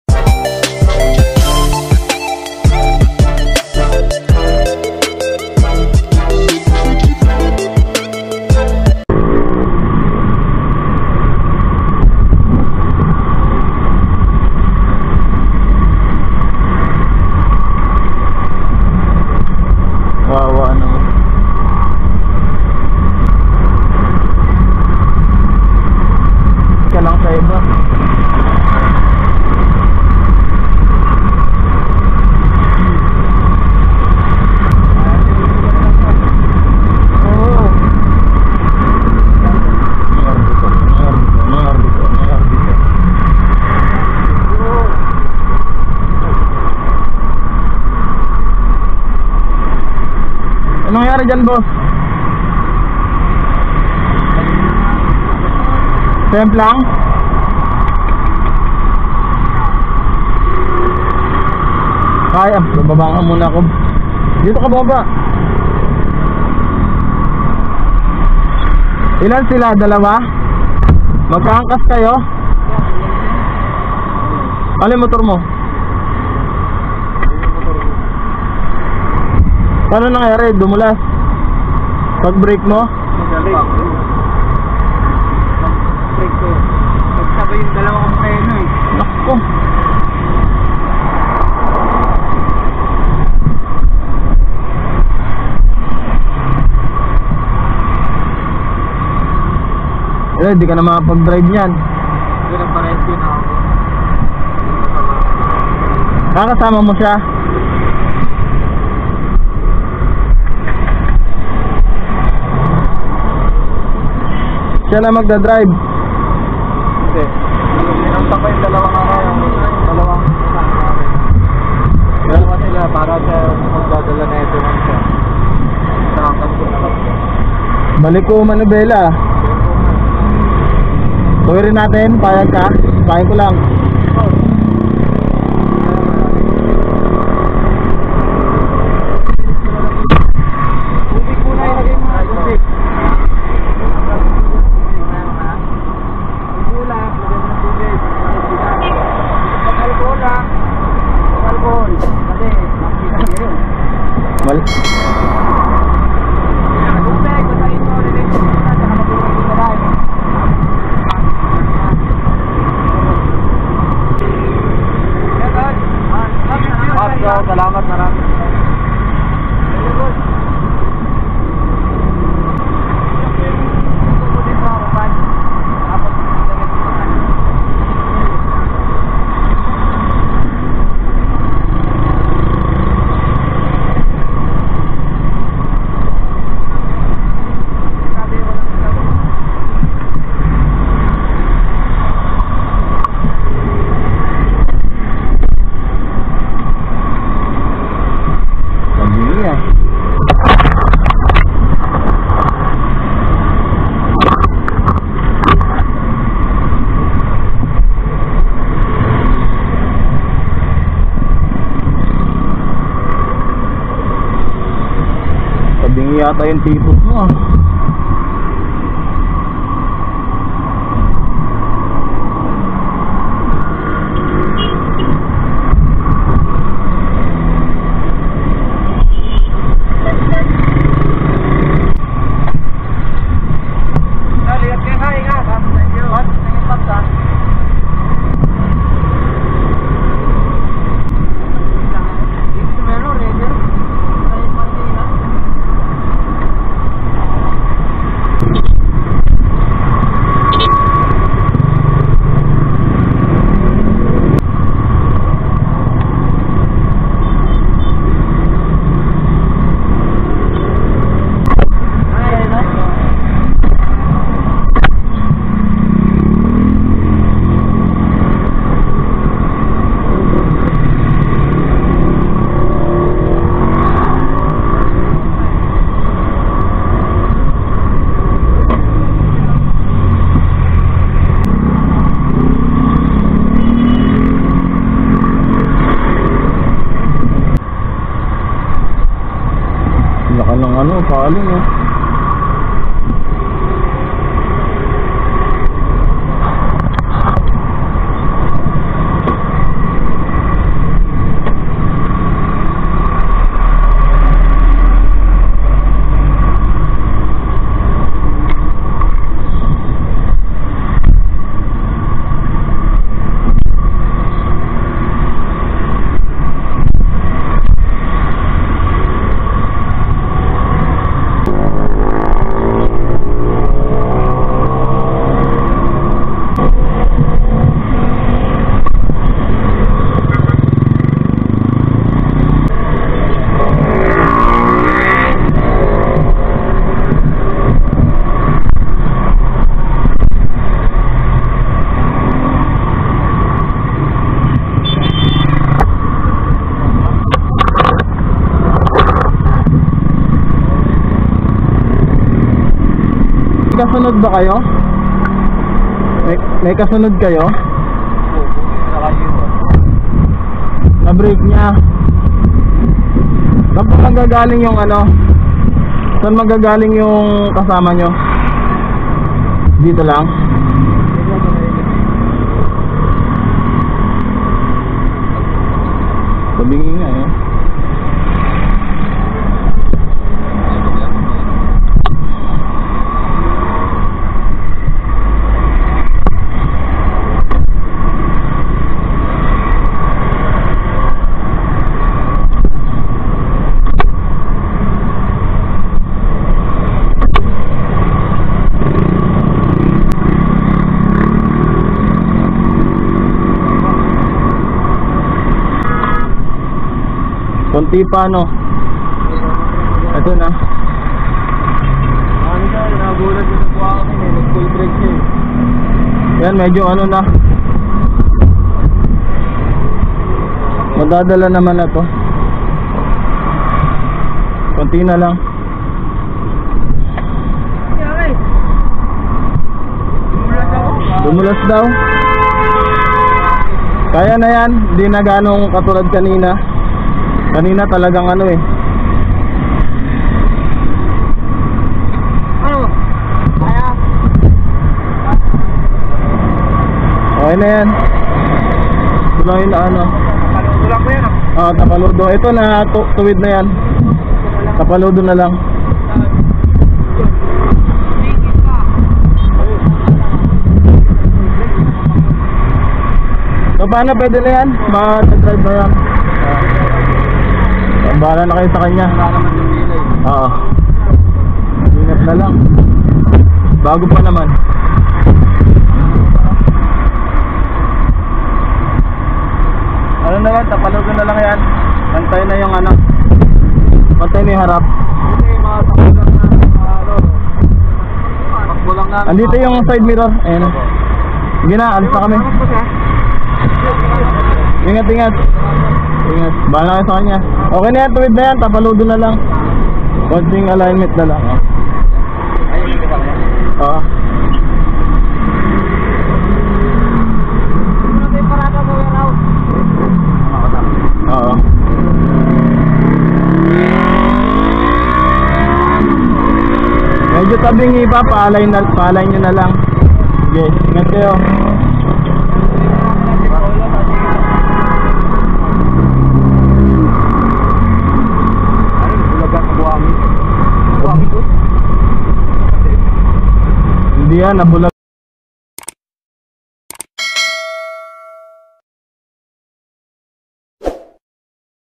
Honda click 150 Accident
7:30pm pauwe na kami ng katrabaho ng makita namin namin ang pangyayari sa taas ng flyover  lagi po tayong magiingat sa daan at wag magmamaneho ng nakainom ng alak.